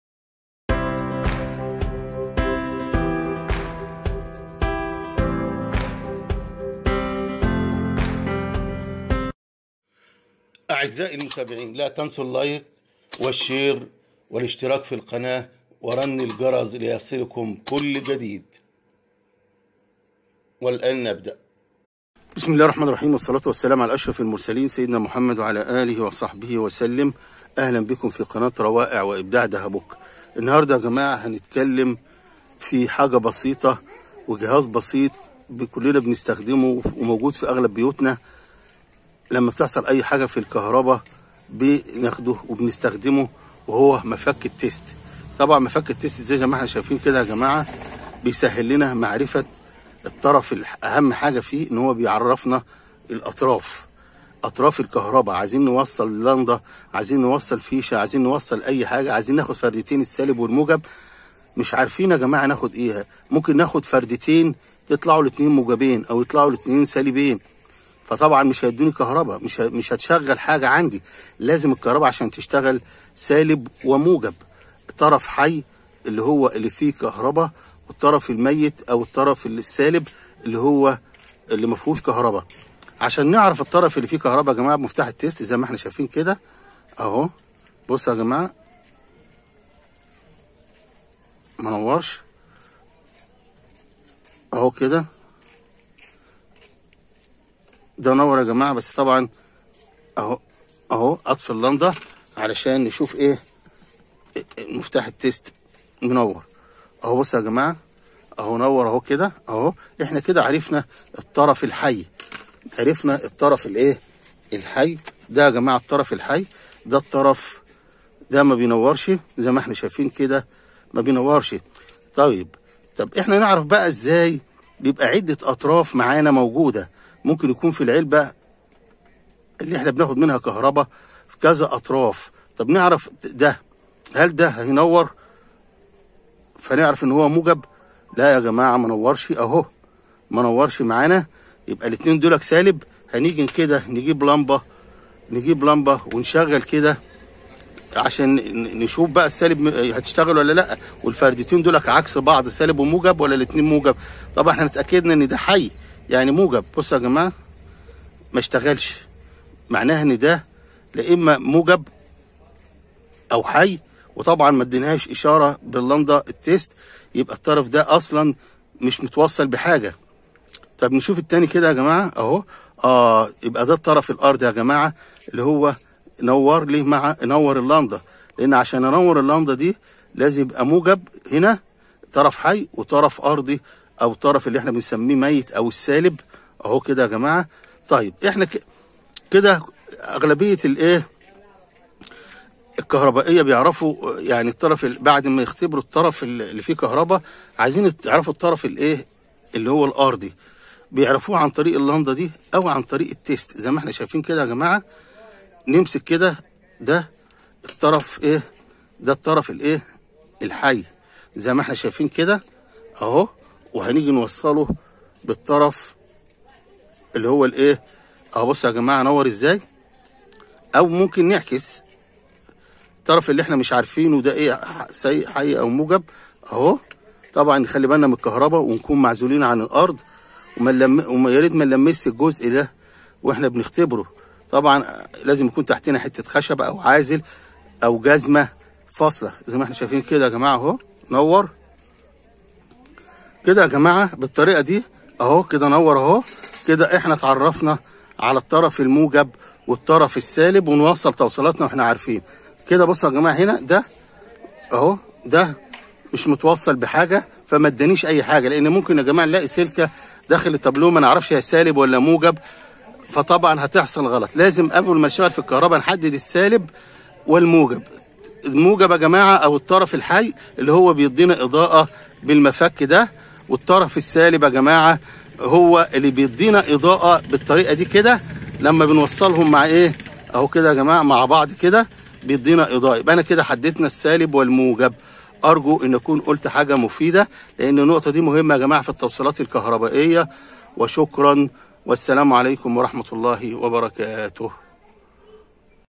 هذا مانجهله عن مفك التست/ الكشف عن سر فى مفك التست
فى هذ الفيديو نتكلم عن إستخدام مفك التست وكيفية إستخداماته والكشف  عن السر الذى يجهله الكثير فى مفك التست
This is what we do not know about the test screwdriver / revealing a secret in the test screwdriver
لو معاك مفك تست شاهد هذا الفيديو
(تحيات قناة روائع وابداع دهببوكا)(لاتنسوا اللايك والاشتراك والتعليق ورن الجرس ليصلكم الجديد)
 رابط قناتى الجديده إبتكارات دهببوكا